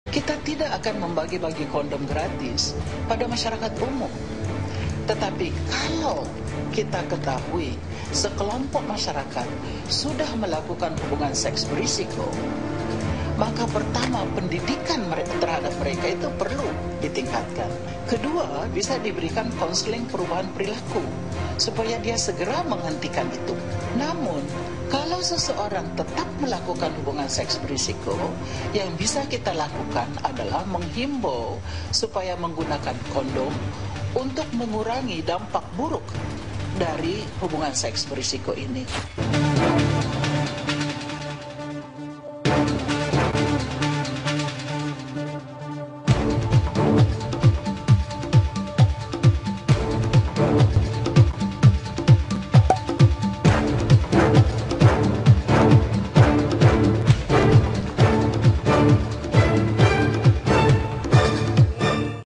Dalih Nafsiah Mboi atas Kontroversi Pembagian Kondom
Inikah alasan Menteri Kesehatan yang baru, Nafsiah Mboi atas putusannya membagikan Kondom Gratis kepada masyarakat ? Cukup Logiskah putusan sang menteri ? Apa fakta dibalik ini semua ?